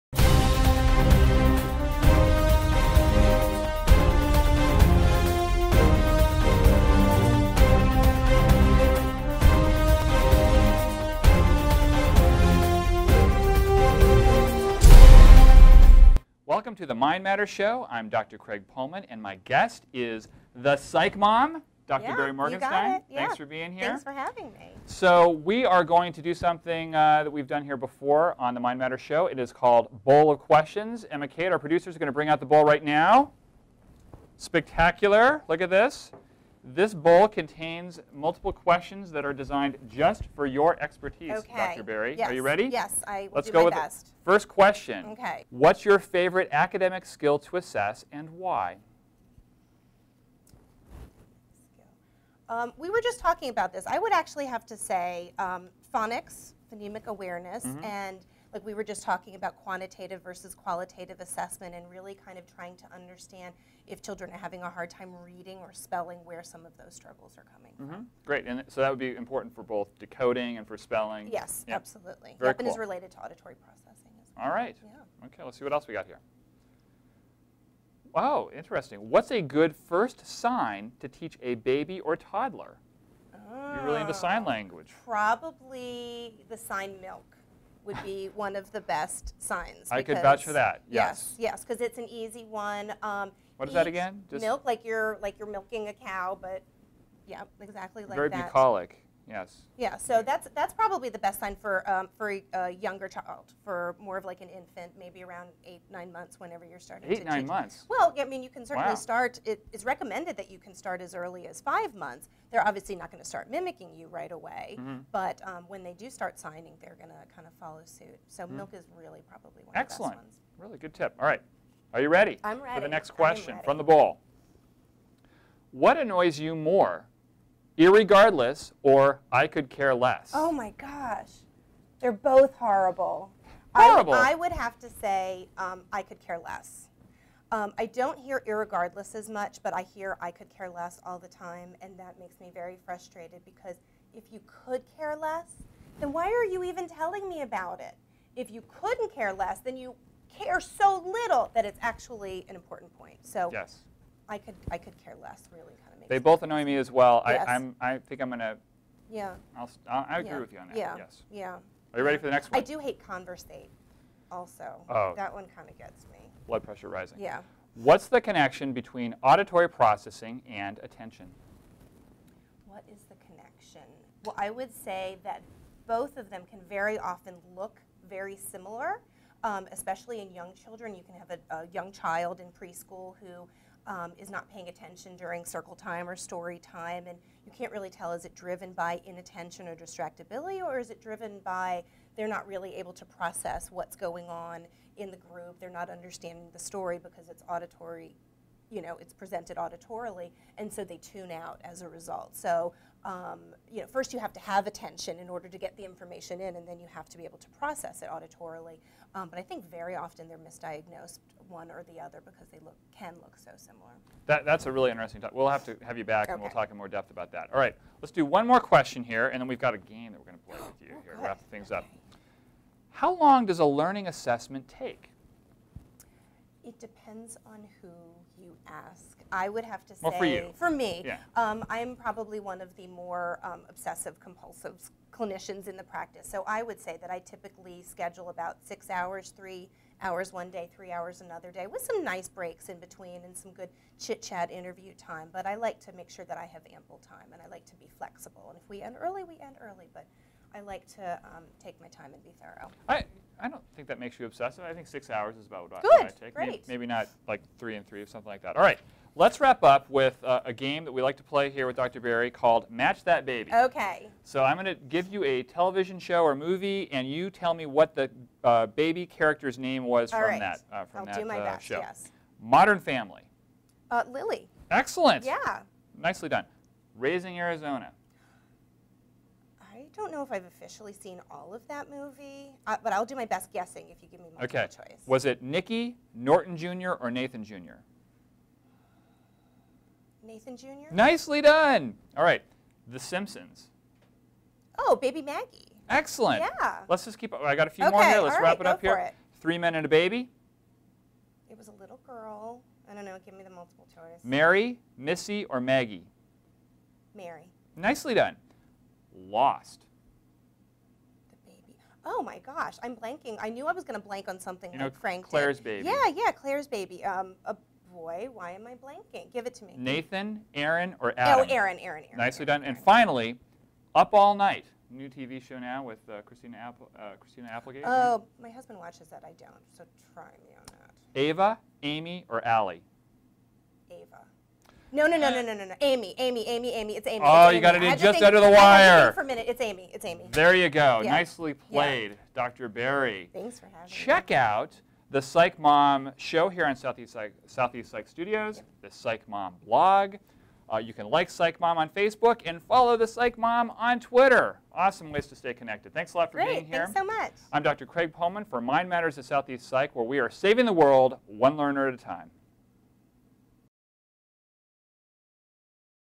The Mind Matters Show - Dr. Barrie Morganstein plays Bowl of Questions
This episode of The Mind Matters Show with Dr. Craig Pohlman features Dr. Barrie Morganstein, The Psych Mom! They play Bowl of Questions to learn more about the Psych Mom.

Follow Mind Matters on Twitter!